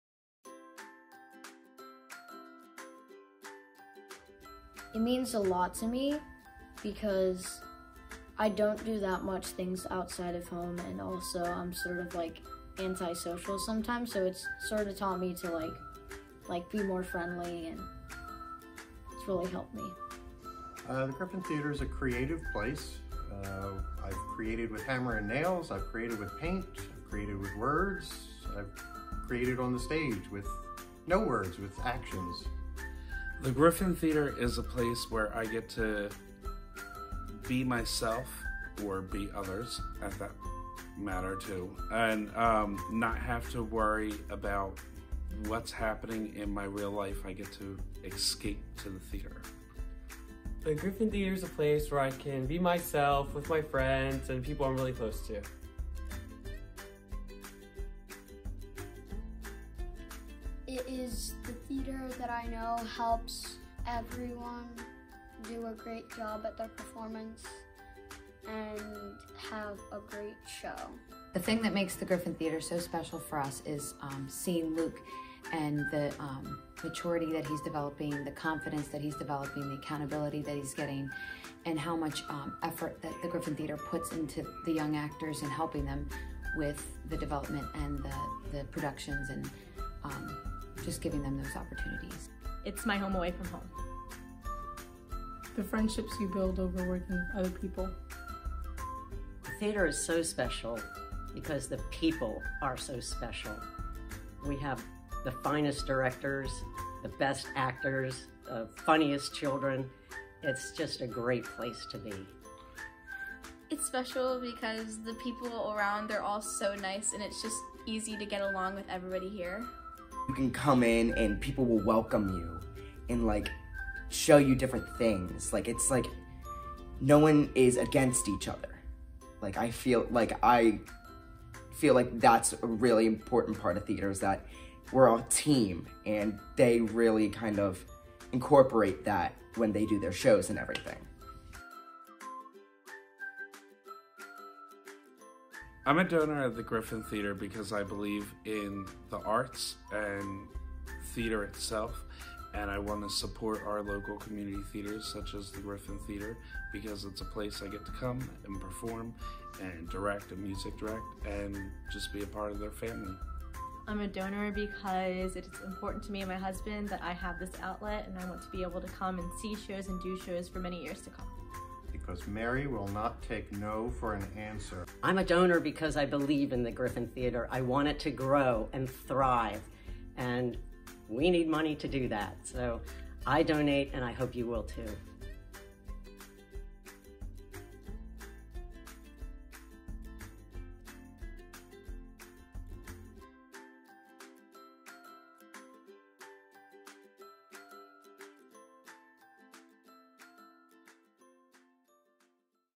The Griffin Theatre--Why is the Griffin Theatre Special?
Watch some of the Griffin Theatre actors, volunteers, and staff explain what the Griffin Theatre means to them, what makes the Griffin Theatre special, and why they are a donor.